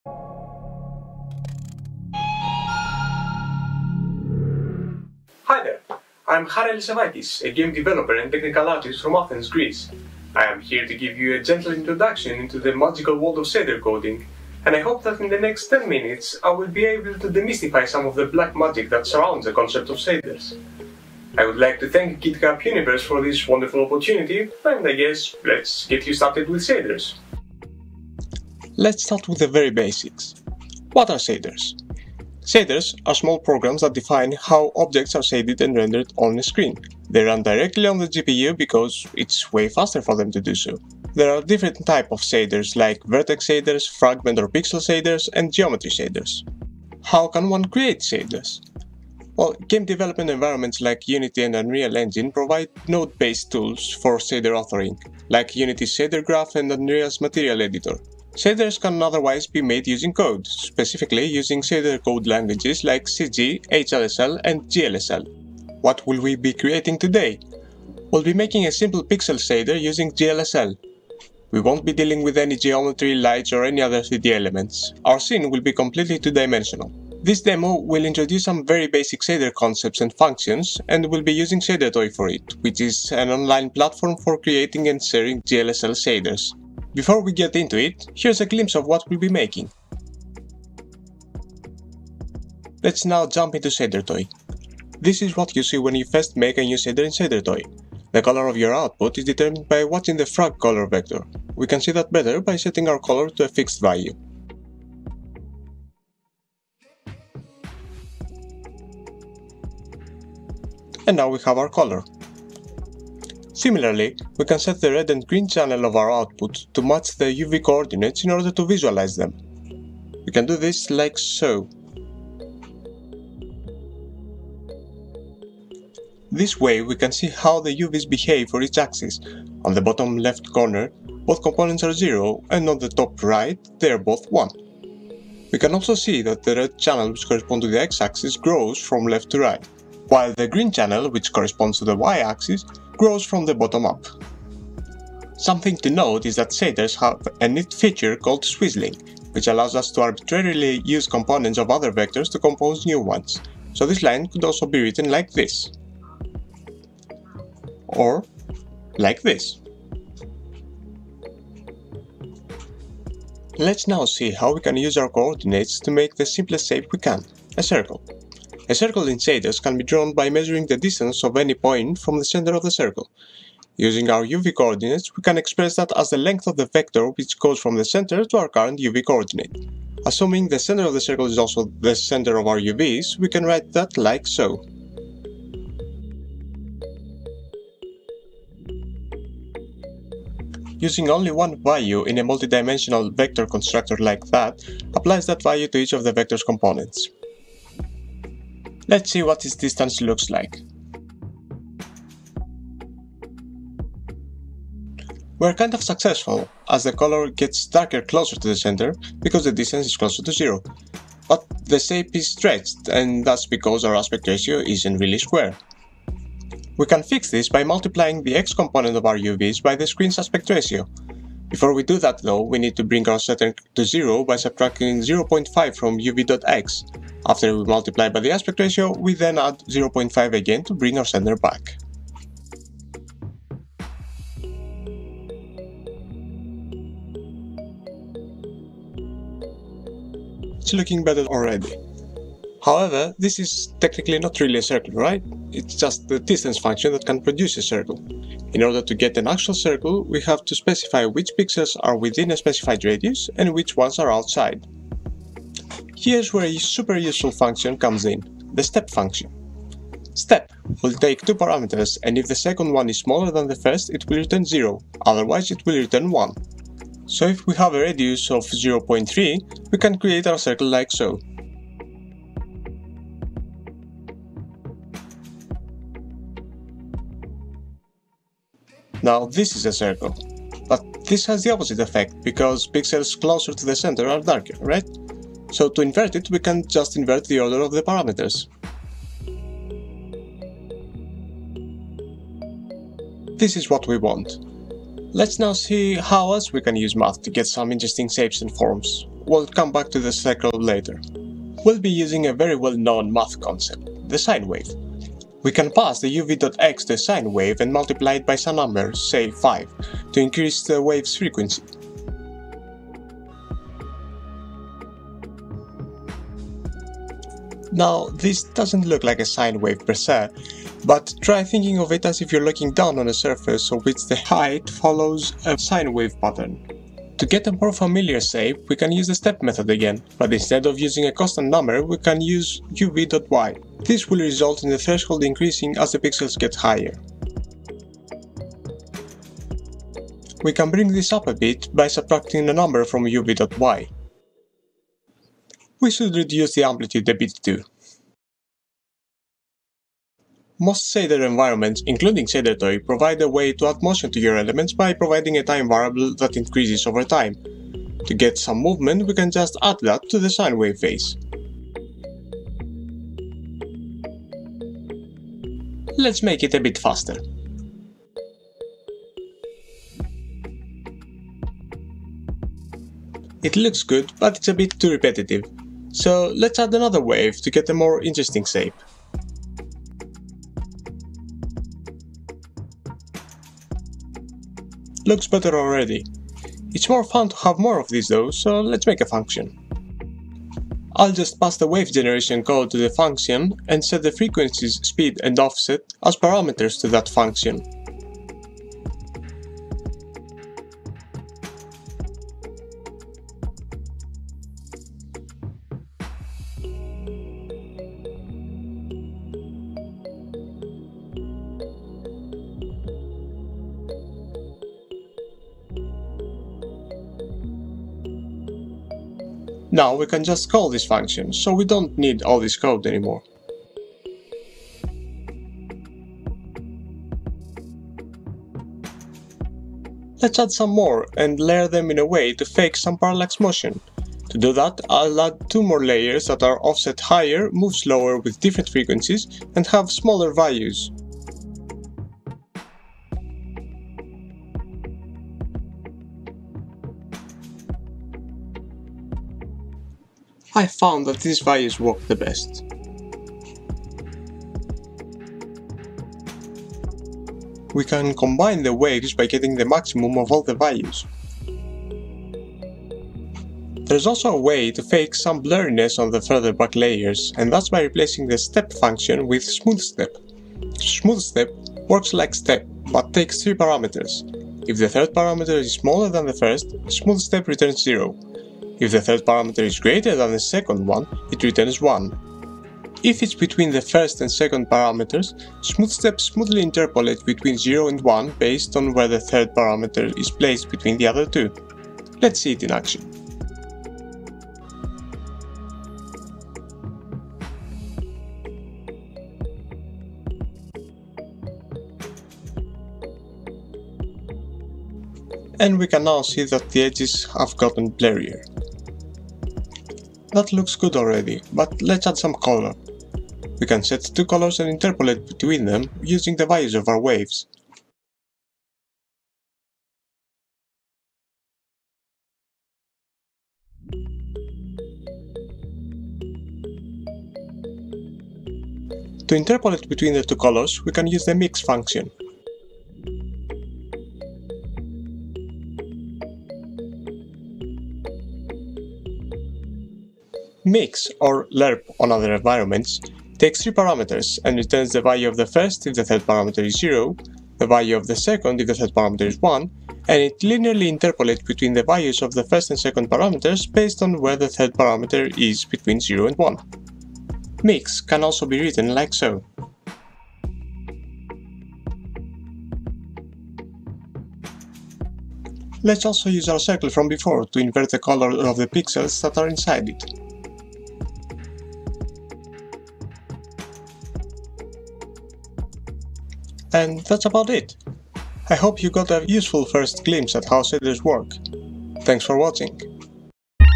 Getting started with Shaders - GitHub Universe 2020
Presented by Harry Alisavakis, Technical Artist, Jumpship

Here you’ll get a short introduction to shaders by going through some of their most basic concepts and by creating a simple visual loop in GLSL and Shadertoy.

Thanks!